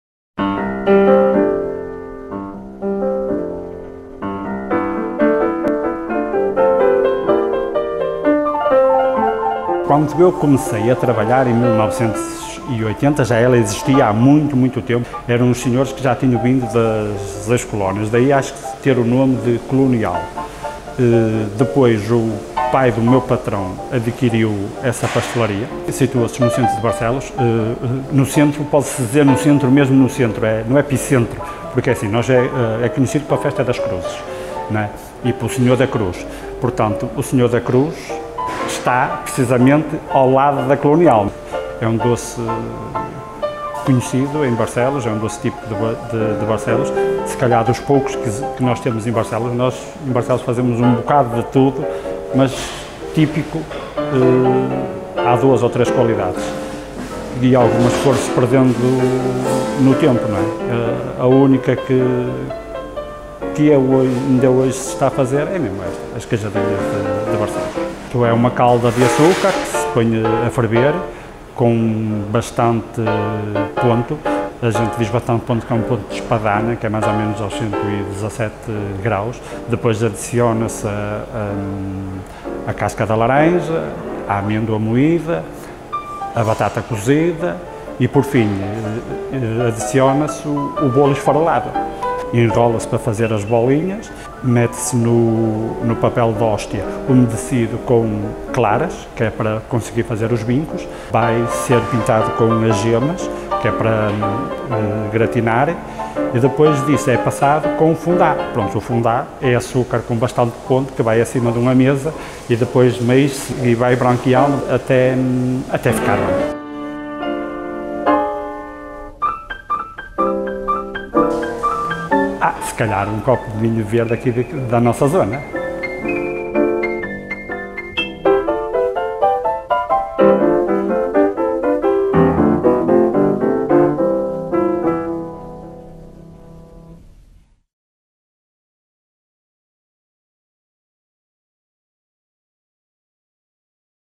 Queijadinhas de Barcelos, Barcelos
Entrevista a / Interview to:
António Barbosa, Confeitaria A Colonial